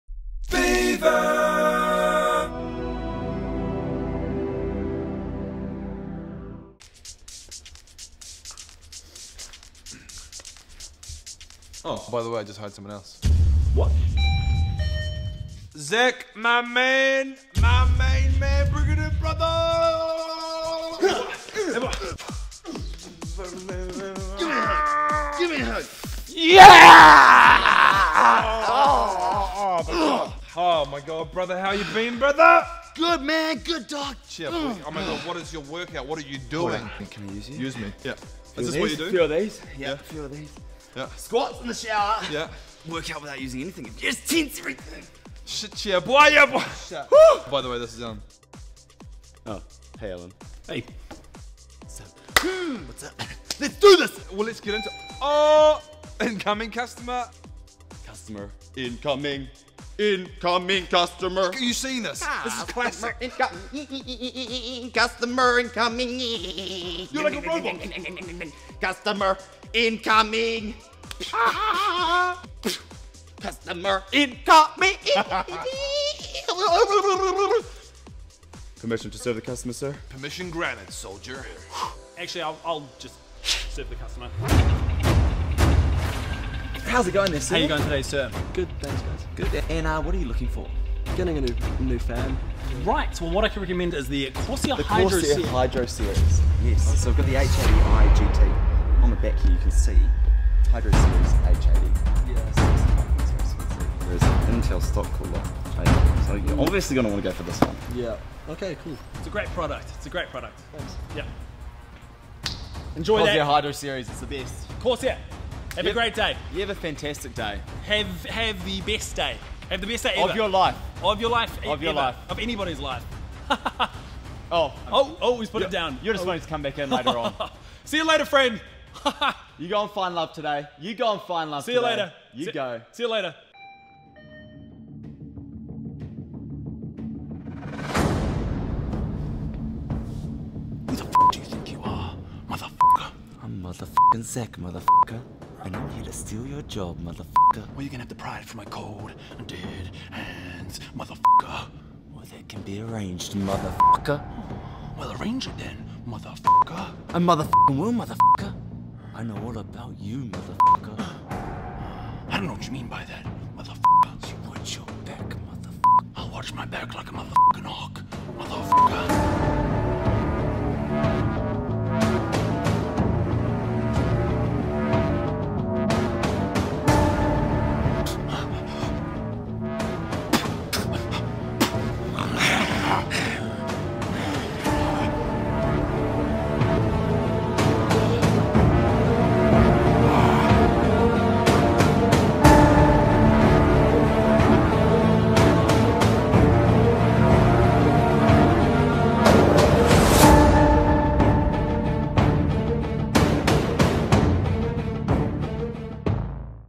The New Employee -
Rowan hires somebody else... lets just say he and Alan don't quite get along. WOAAAAAAAAH!